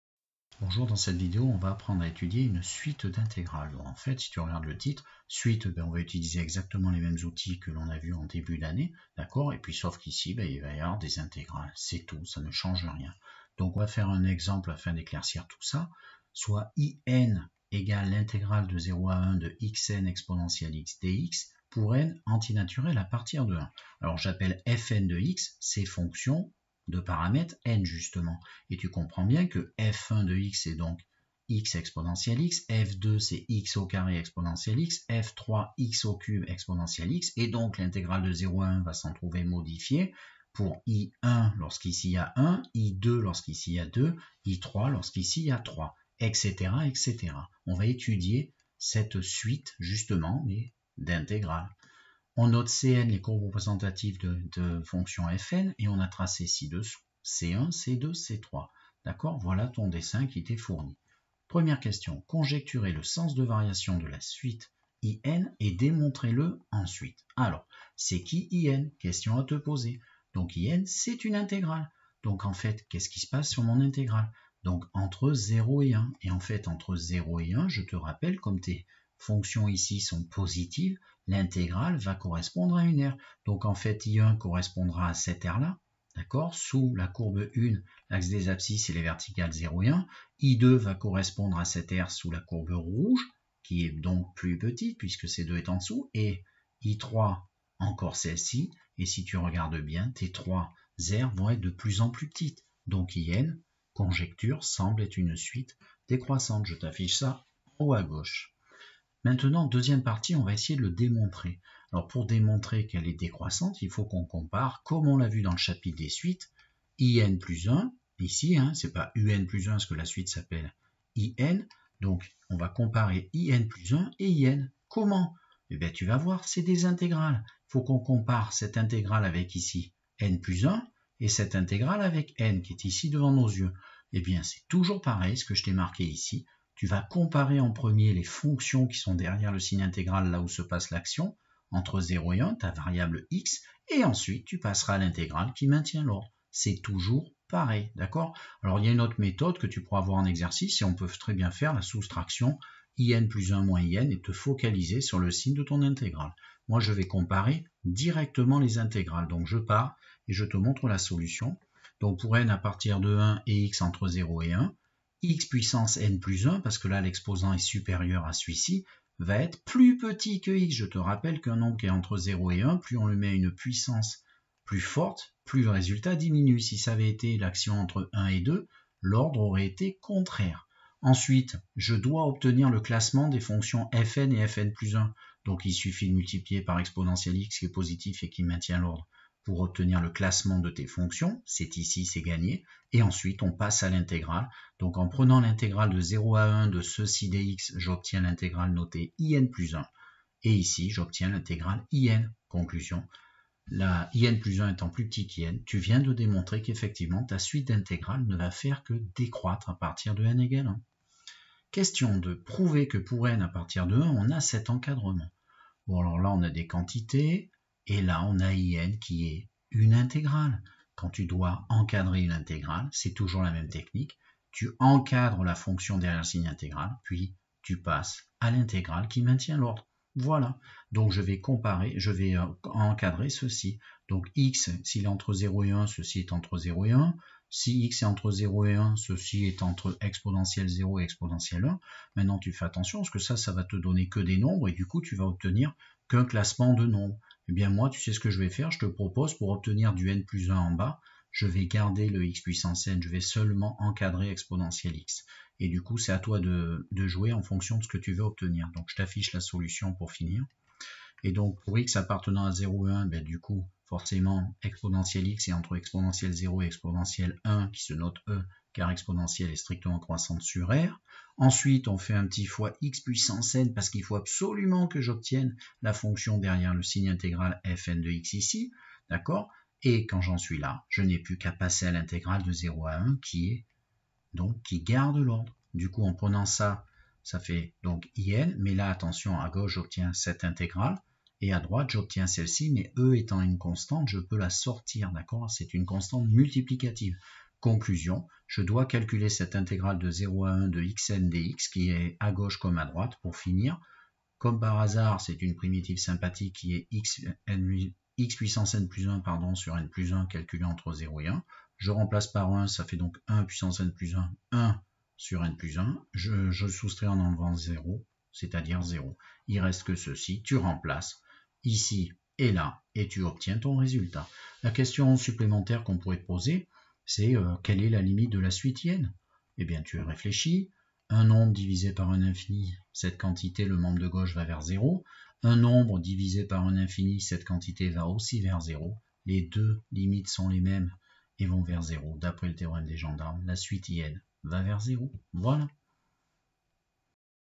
SUITE D'INTEGRALES. Terminale spécialité mathématiques.
Apprendre à étudier une suite d'intégrales. Les indispensables en moins de 5 minutes en terminale spécialité mathématiques.